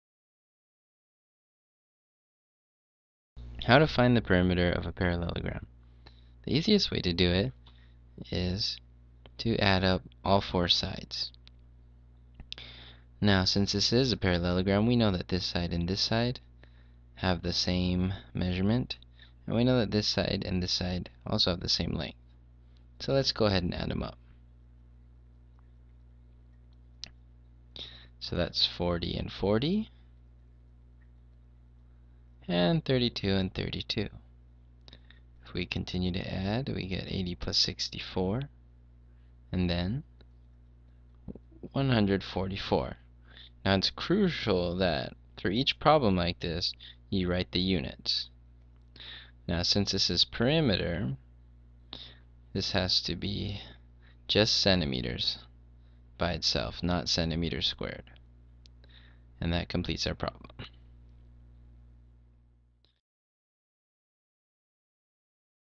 How to Find the Perimeter of a Parallelogram
- How to find the perimeter of a parallelogram.  For more practice and to create math worksheets, visit Davitily Math Problem Generator